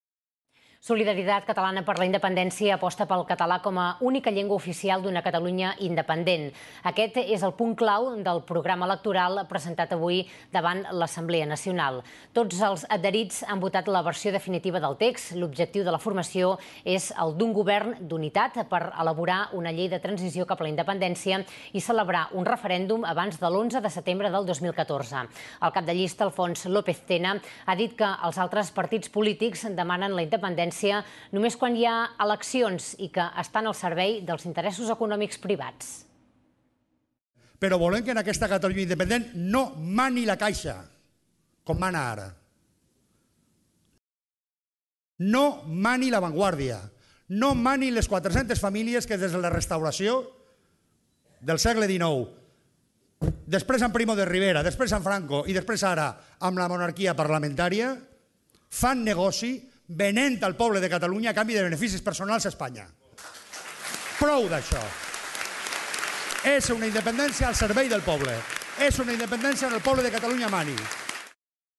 López Tena exigeix regeneració democràtica (04-11-2012).
López Tena exigeix regeneració democràtica en l'Assemblea de SI que aprova el programa electoral (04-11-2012)

[SOLIDARITAT 2.0]